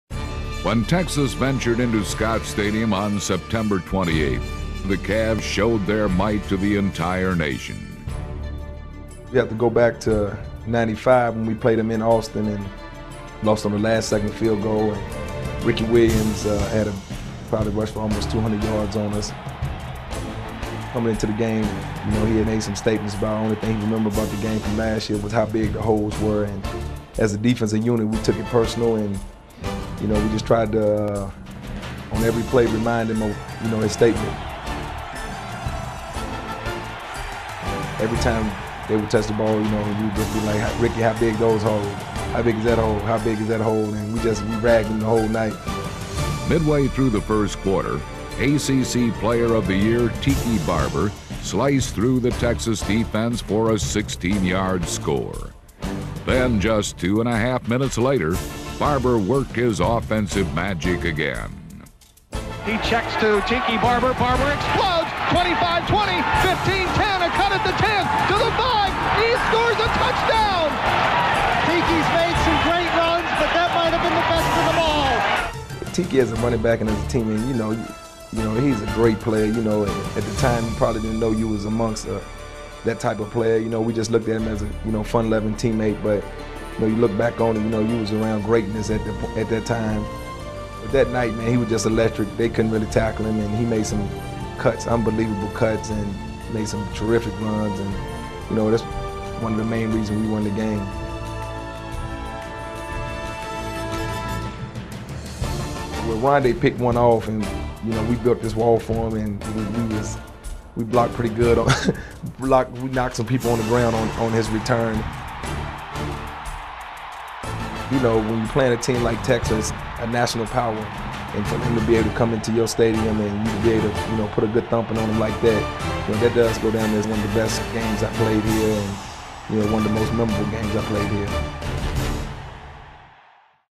Football Feature - Texas
Led by Poindexter, Farrior, Sharper and the Barbers, Virginia pounds Texas 37-13 in 1996.